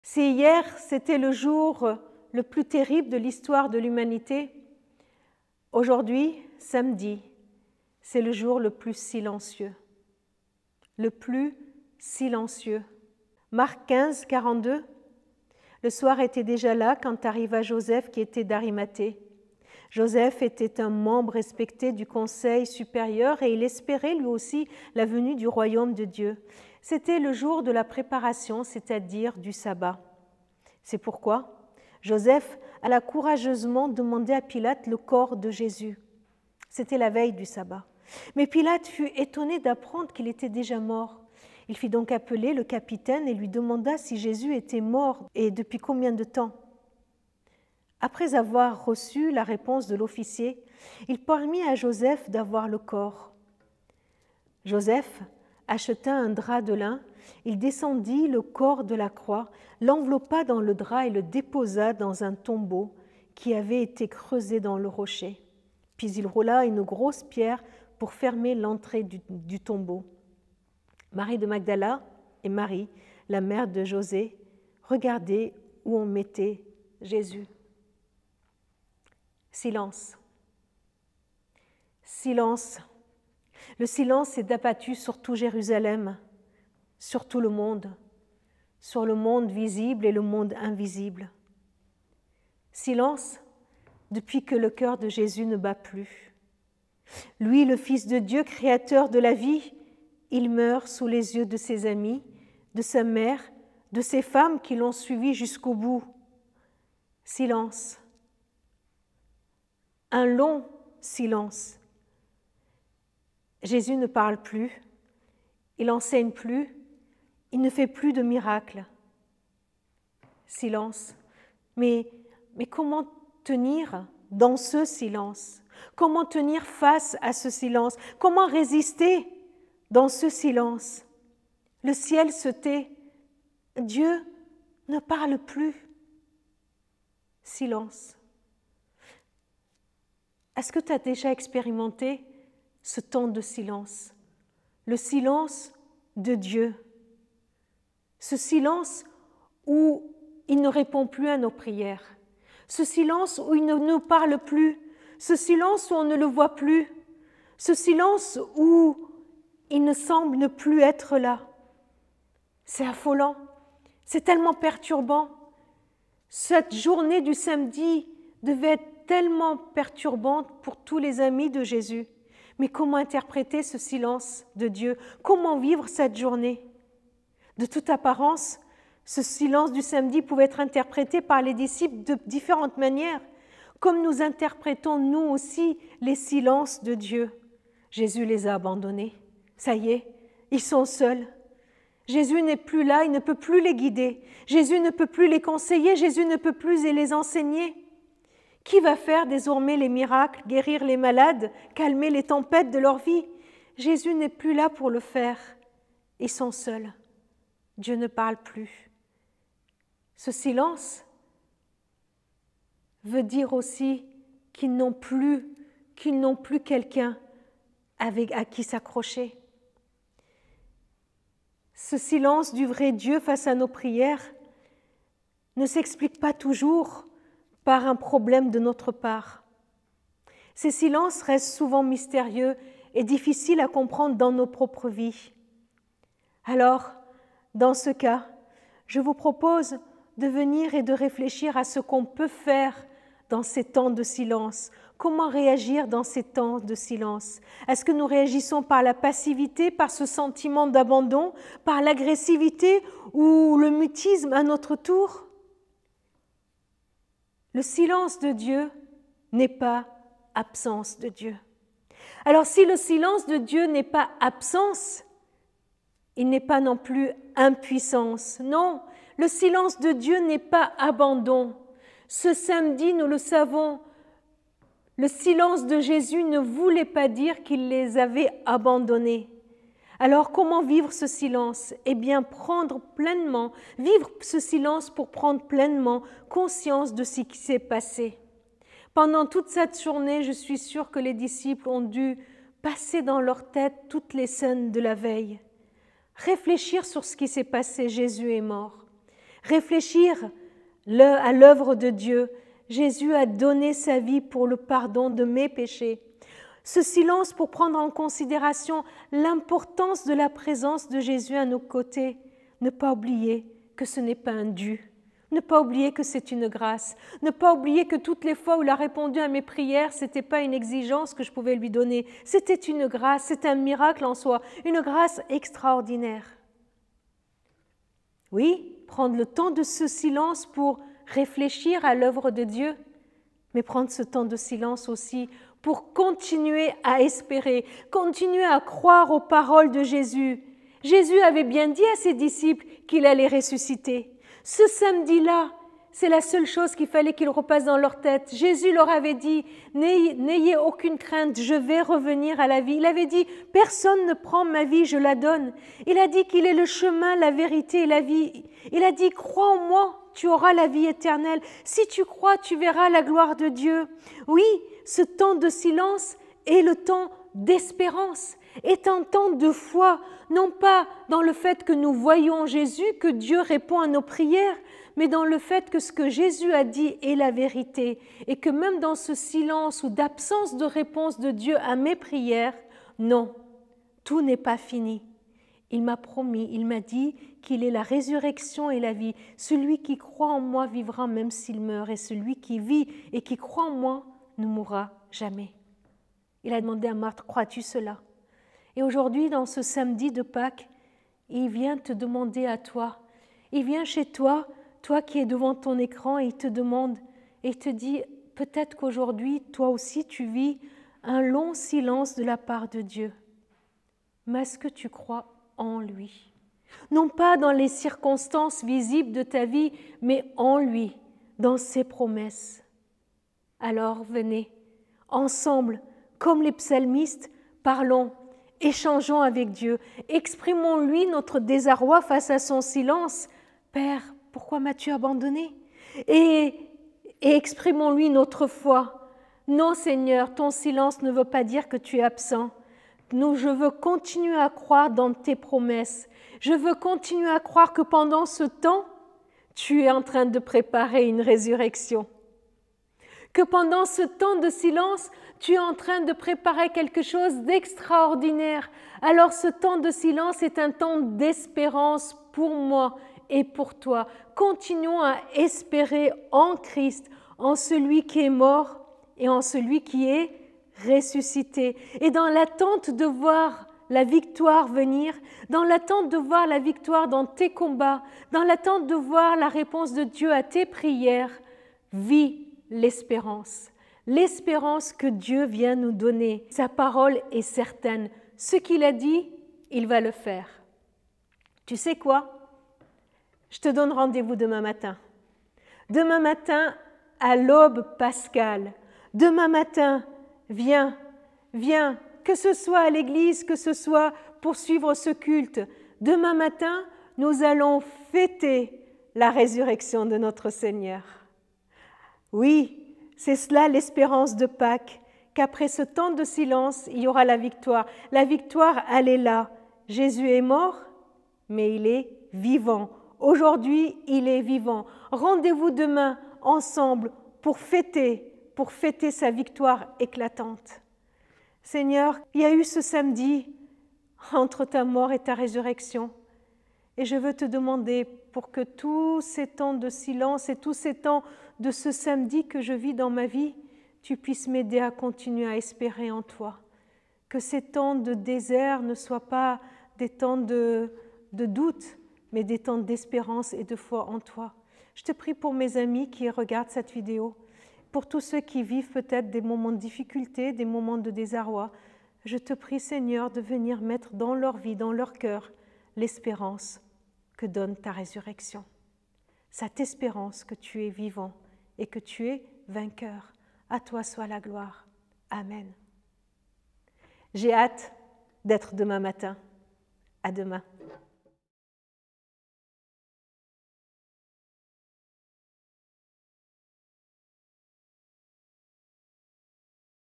Pâques Méditation 6 - Silence - Marc 15.42-47 - Jéma Taboyan - Église M
Silence

Texte Biblique : Marc 15.42-47 (Version BFC)
Méditation apportée par Jéma Taboyan

Jésus pousse son dernier soupir et l’univers entier est plongé dans le silence. Le silence de Dieu. Comment vivre ce silence là, même dans nos vies ? Certes, le silence de Dieu ne signifie pas son absence de Dieu et nous oblige alors à vivre l’espérance.